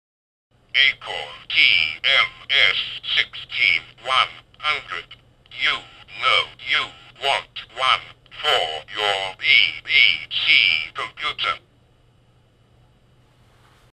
Acorn Speech PHROM TMS6100 Emulator V2
A demonstration of the TMS6100 speech PHROM (PHrase Read Only Memory) for use with the TMS5220 Voice Synthesis Processor in an Acorn BBC Microcomputer.